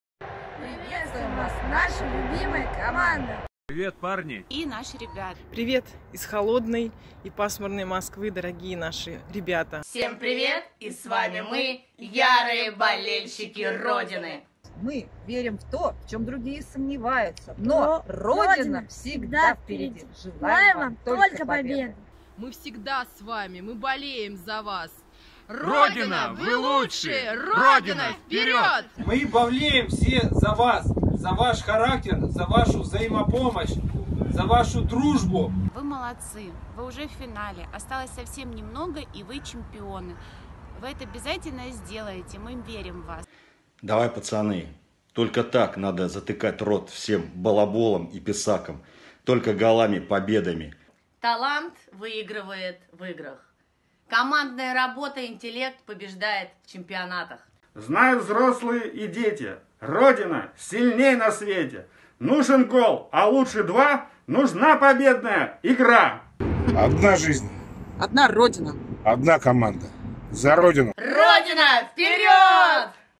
Вперёд Родина
Верим в Вас! Для нас команда уже победила и всем всё доказала!
 Возвращайтесь скорее, нам без разницы с какой медалью и кубком мы Вас сильно любим!!!